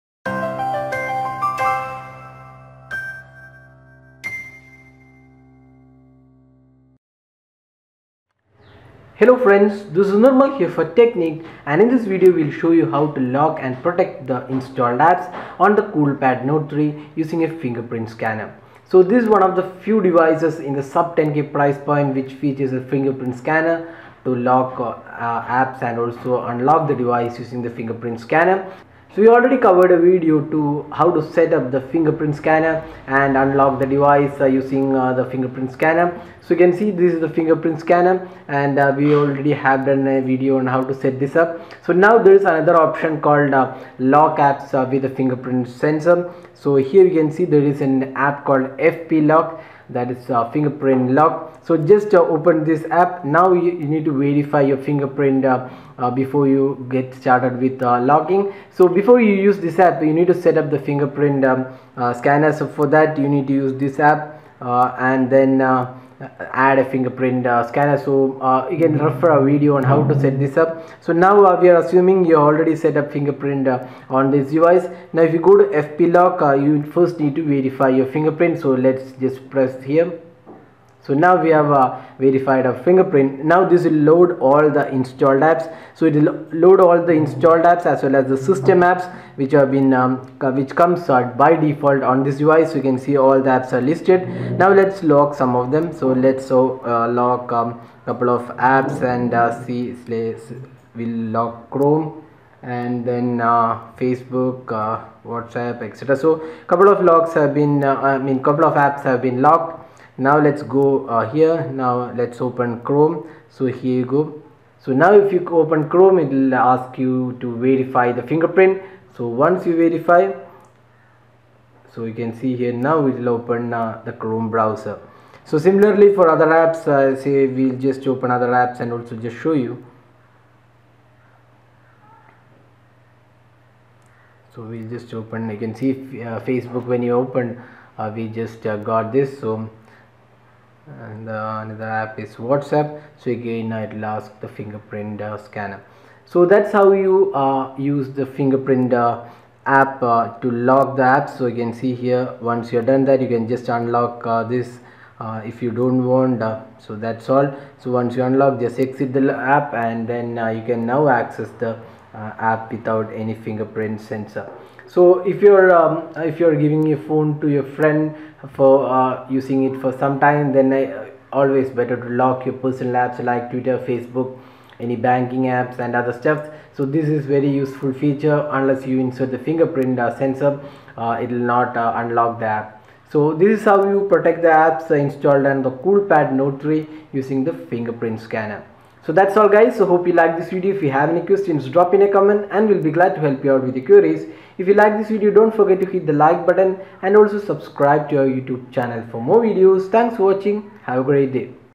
How to Lock Apps on Coolpad Note 3 using Fingerprint Scanner
Coolpad Note 3 is the first device to feature fingerprint scanner below price of Rs 10000 and here is a quick video on how to use the fingerprint scanner to lock and protect apps.